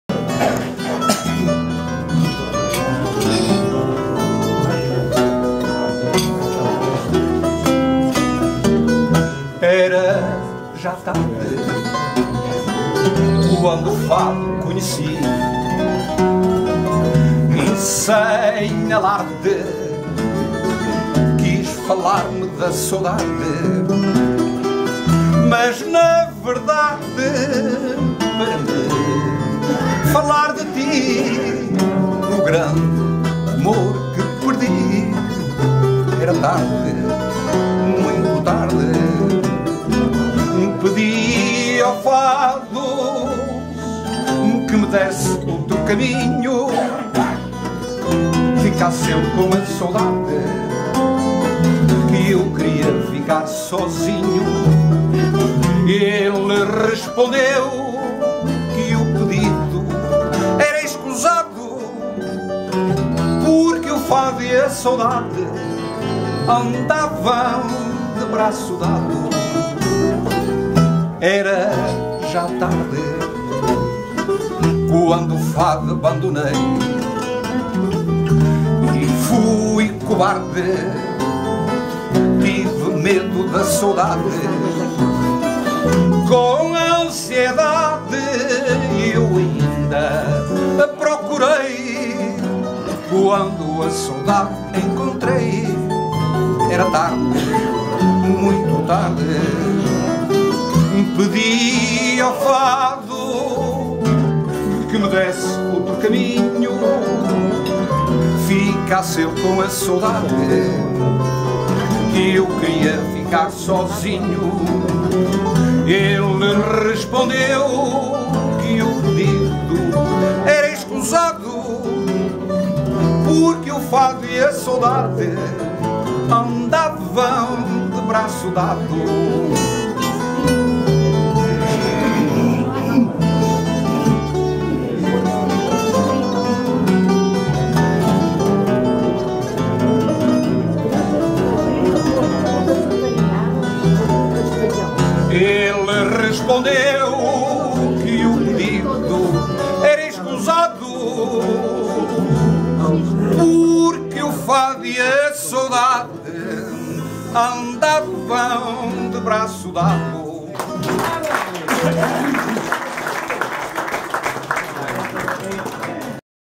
NANO VIEIRA FADO , NO RESTAURANTE SÃO JOÃO , EM SÃO COSME GONDOMAR
O FADISTA , NANO VIEIRA , ACTUOU ,NO RESTAURANTE SÃO JOÃO ,EM SÃO COSME GONDOMAR  E  FOI ACOMPANHADO  A GUITARRA DE FADO POR,EDUARDO JORGE,  E A VIOLA DE ÂNGELO JORGE ,E NO CONTRA BAIXO , TONY GUIMARÃES  COM JORGEFERREIRAFADO ,  PARA OS QUATRO CANTOS DO MUNDO SEM FINS LUCRATIVOS , .EM TUDO ISTO E FADO